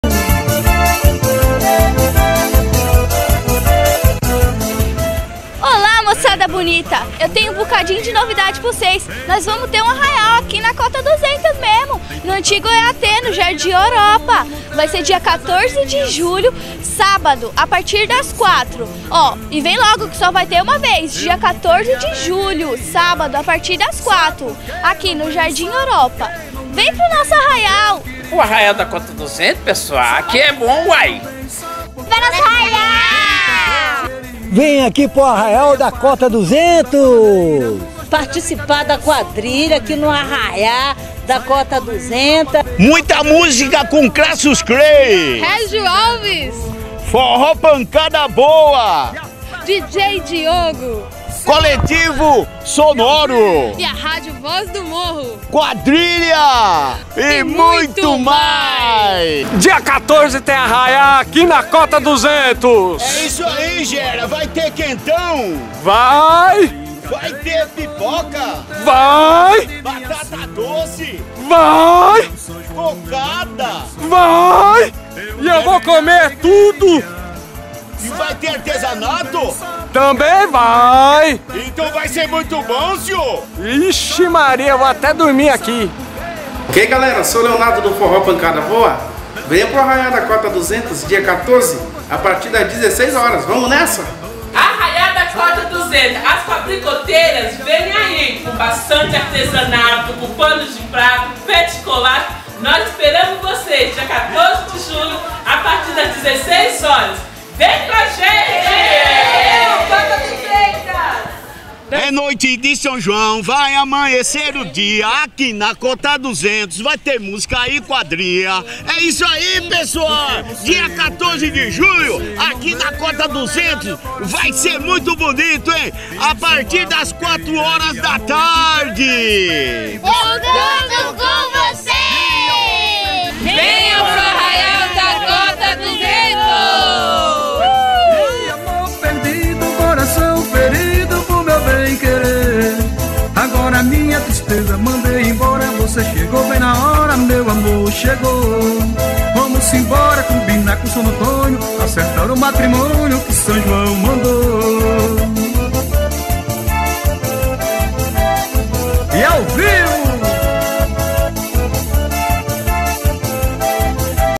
Chamada Festa Julina 2012 - Bairros Cota
O PROJETO COMCOM CONVIDA A TODOS PARA  PARTICIPAREM DA FESTA JULINA QUE ACONTECE DIA 14-07 NO JARDIM EUROPA NA COTA 200. VENHA PARTICIPAR VOCÊ TAMBÉM!